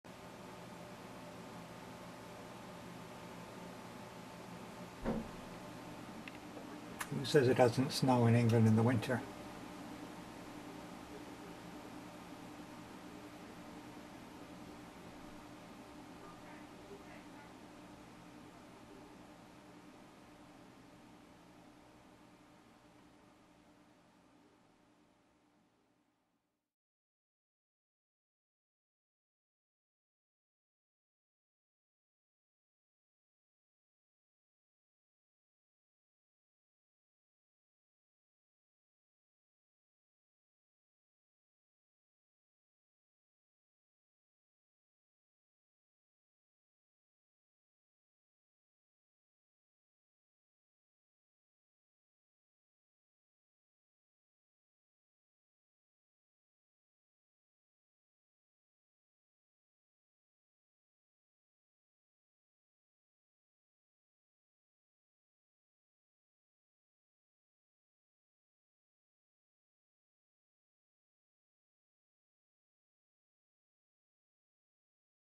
Snowy Winter
Scenes showing Snowy, the white West Highland Terrier (Westie) on a cold, snowy winter's day in Essex in February 2010